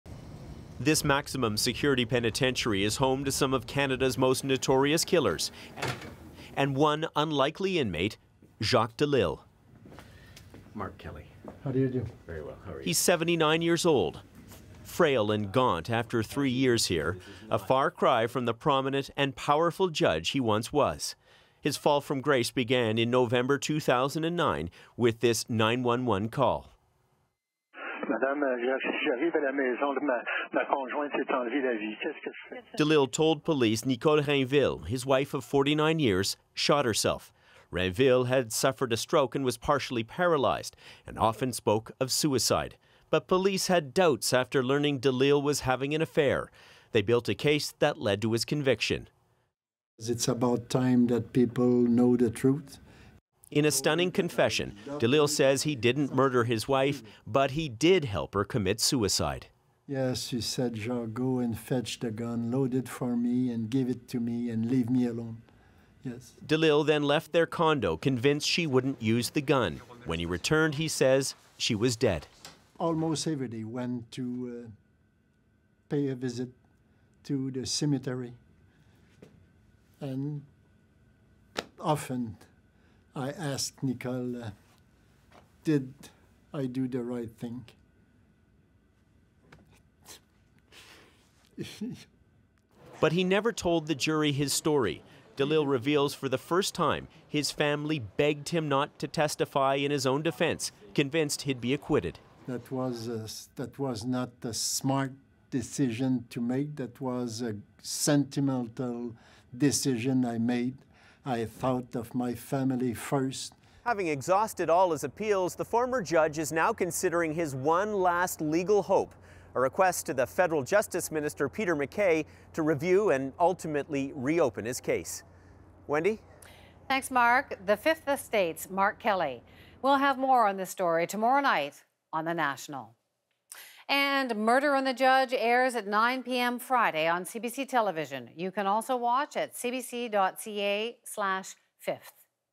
Jacques Delisle, ex-judge convicted of wife's murder, says he didn't do it
Jacques Delisle, the only judge in Canadian history to be convicted of murder, says he is not guilty of killing his wife.